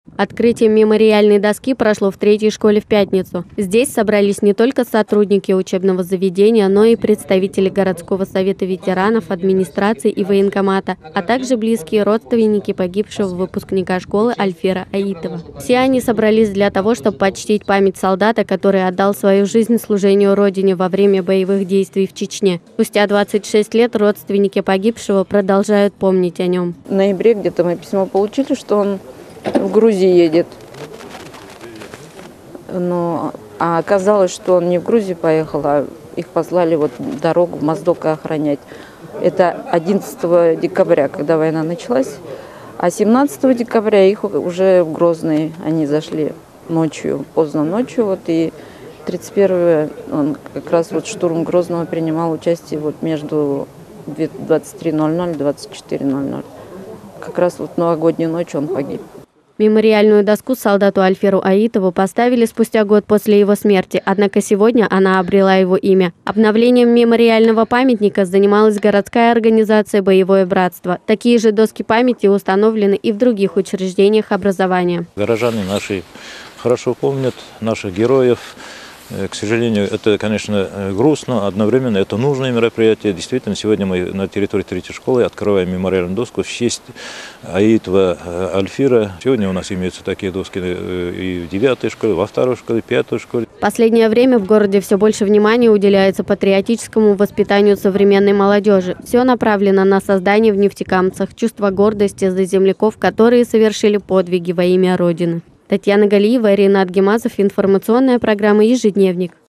Увековечили память выпускника
В третьей школе состоялось открытие мемориальной доски